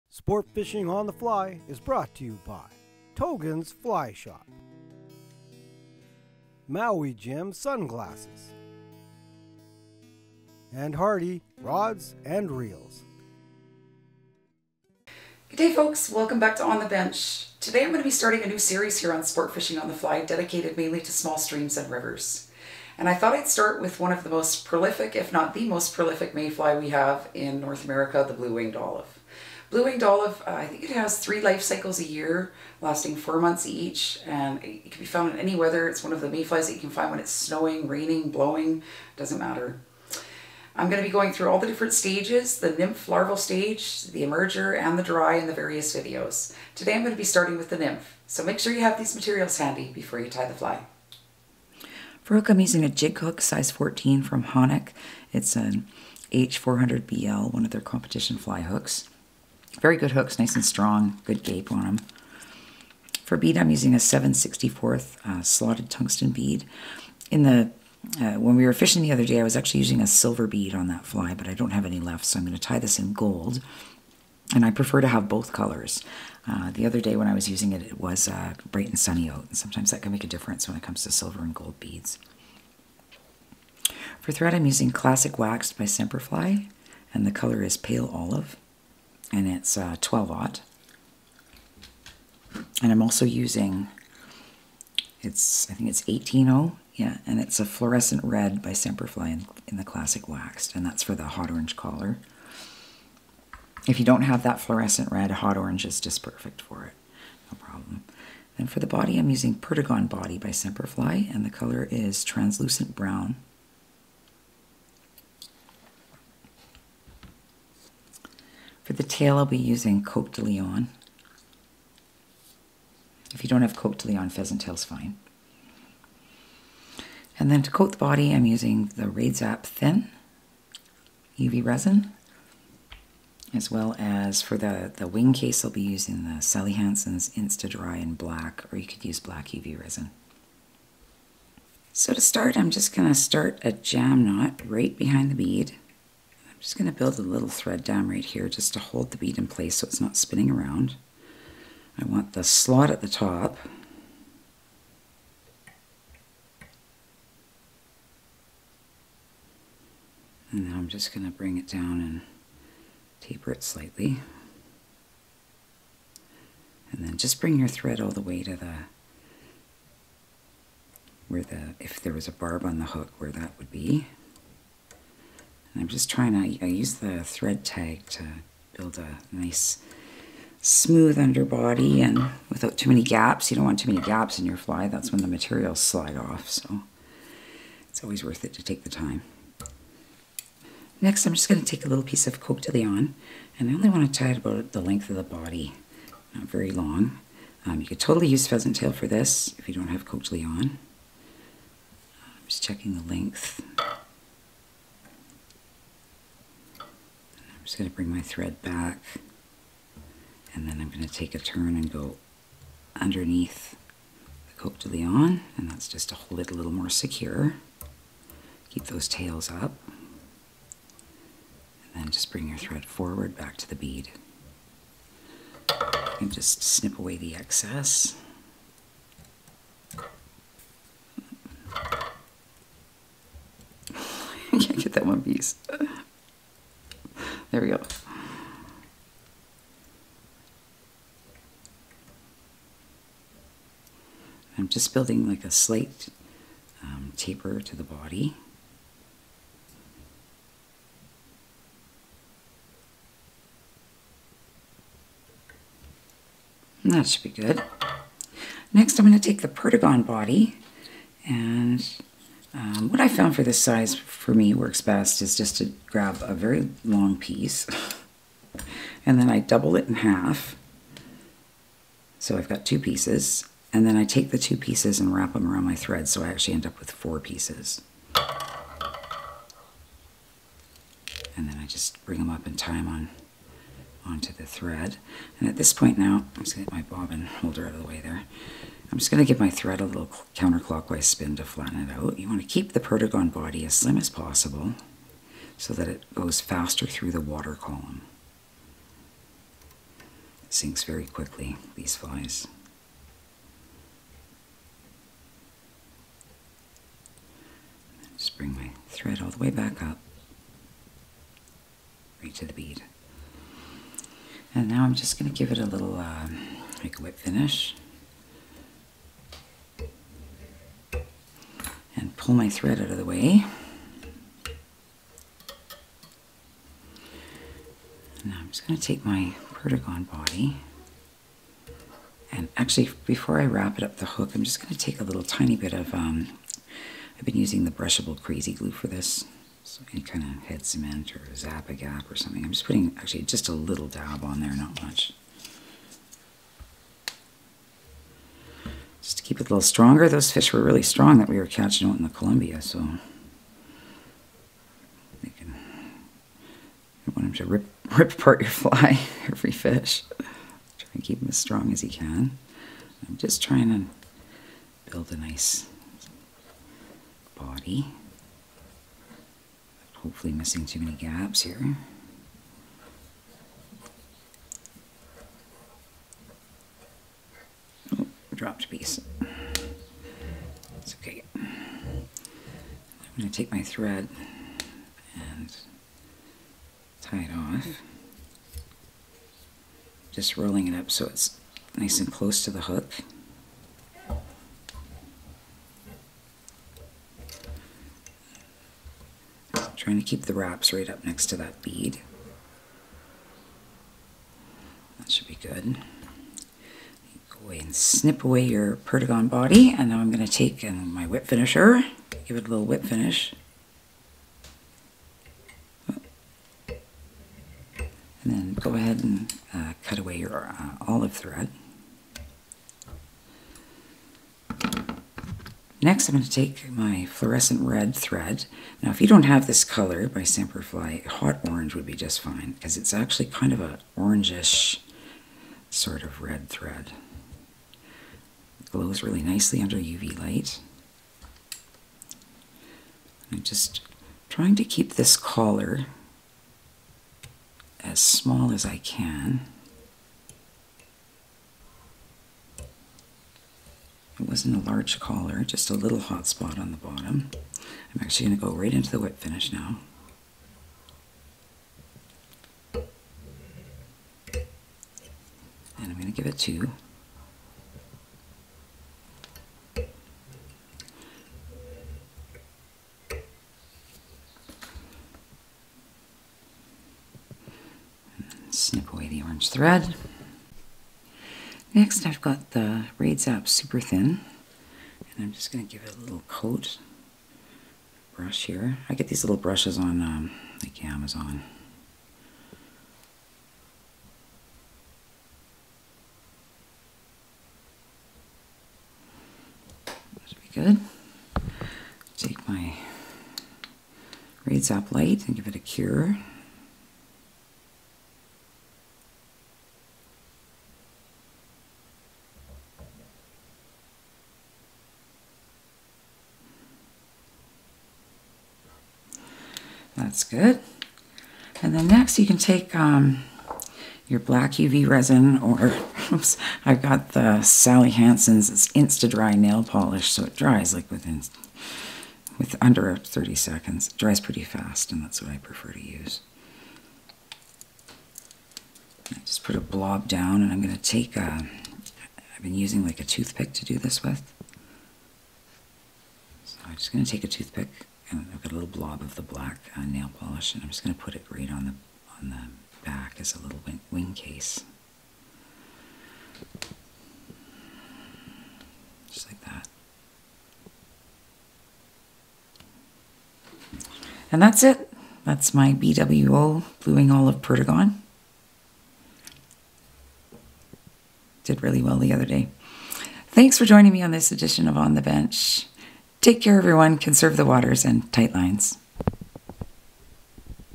FLY TYING: BLUE WING OLIVE PERDIGON NYMPH
Deb Paskall ties an excellent jig style nymph that is one of the MOST effective nymph patterns ever created. The Perdigon matches many insect nymph hatches and is easy to tie. Must have for your fly box.

Hook: Hanak H 400 BL
Bead: 7/64 Tungsten Silver or Gold
Threads: Semperfli Classic Waxed 12/0 Pale Olive and 18/0 Fl Red
Tail: Coq de Leon
Body: Semperfli Perdigon Body Translucent Brown
Coating: UV Resin Raidzap Thin
Wingcase: Sally Hansens Insta Dry Black